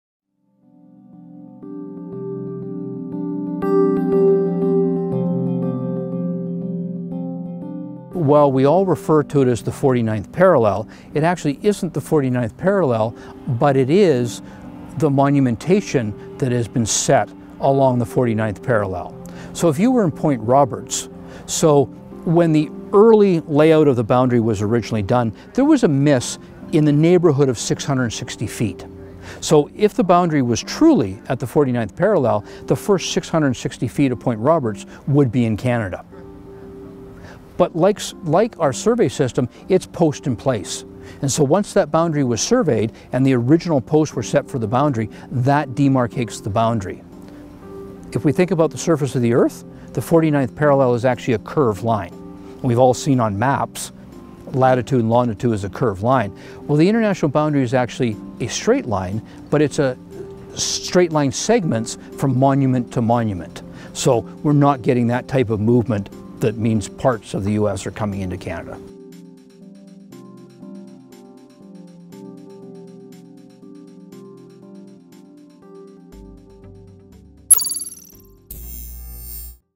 Land Surveying terms: The 49th Parallel
What is the 49th Parallel and how is it used in land surveying?